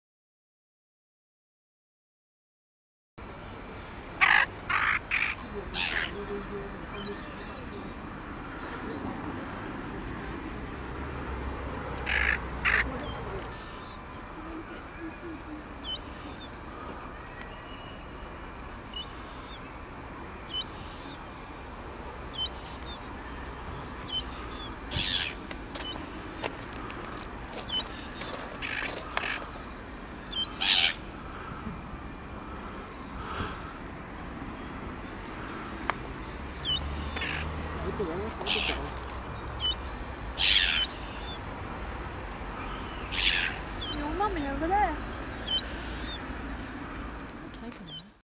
The Crow and the Seagull
A rather Cocky Crow that has taken a dislike to Seagulls